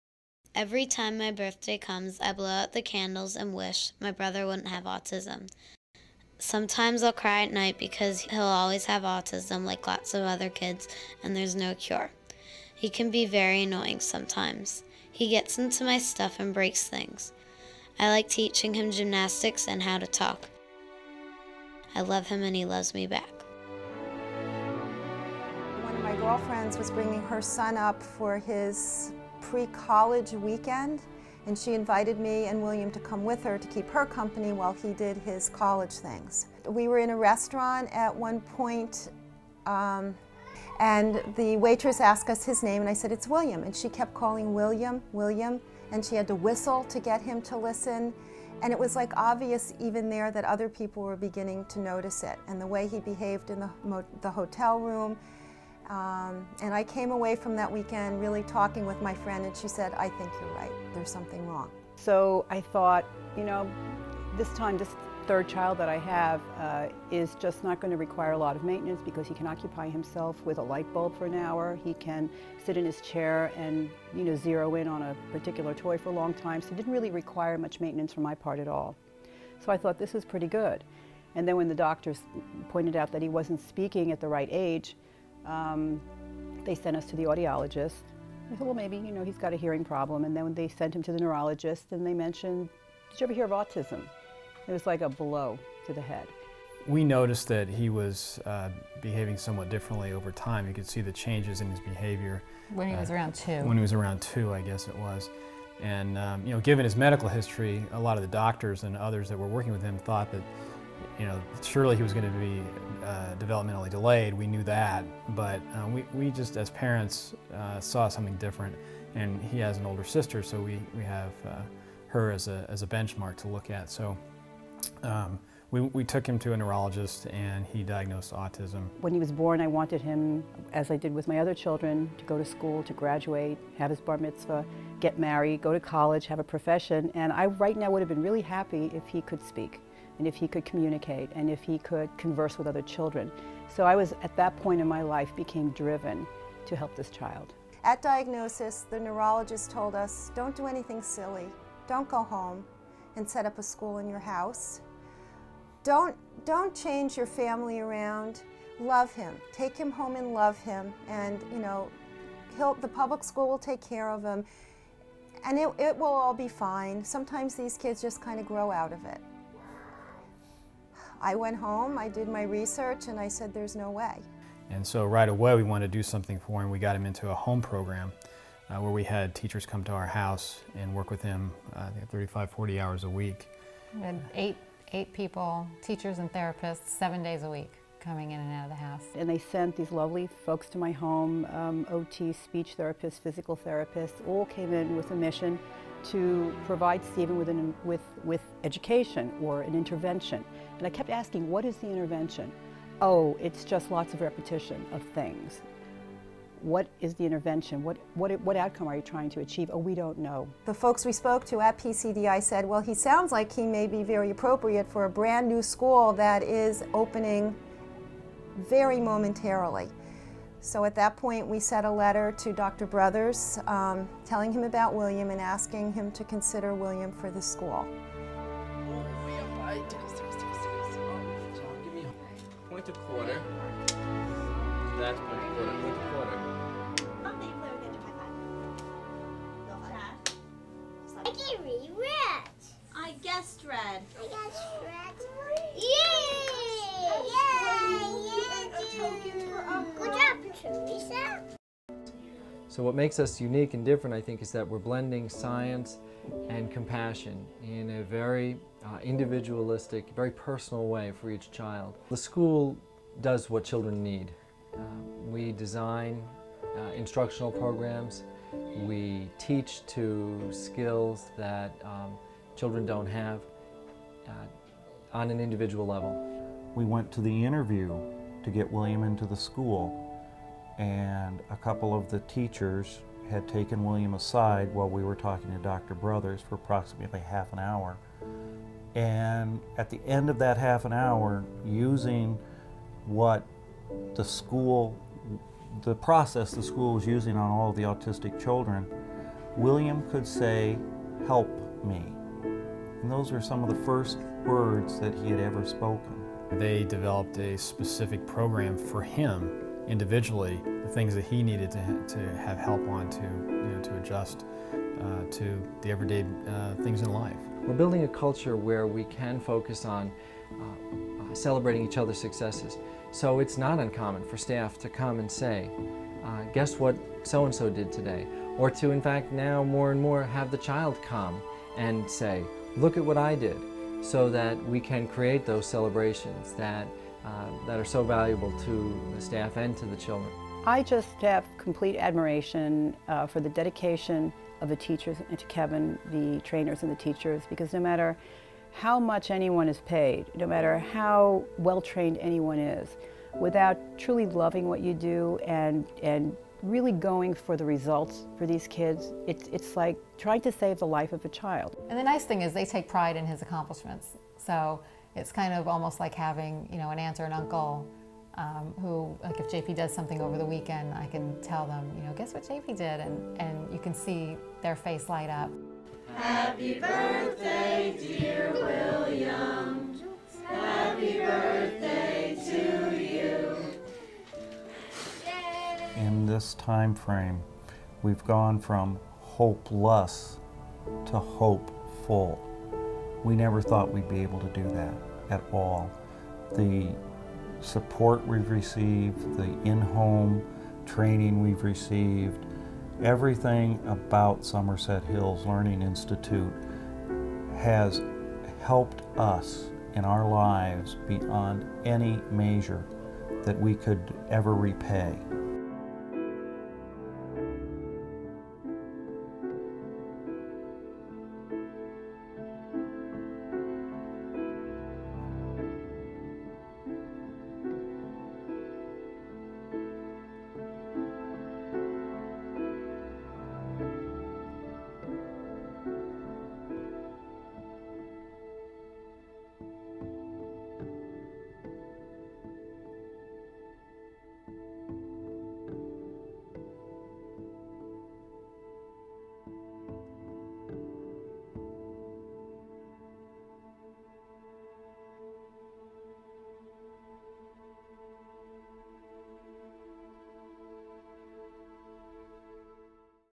Somerset Hills Learning Institute
Hope. This piece defines what hope means for children and parents of children with Autism.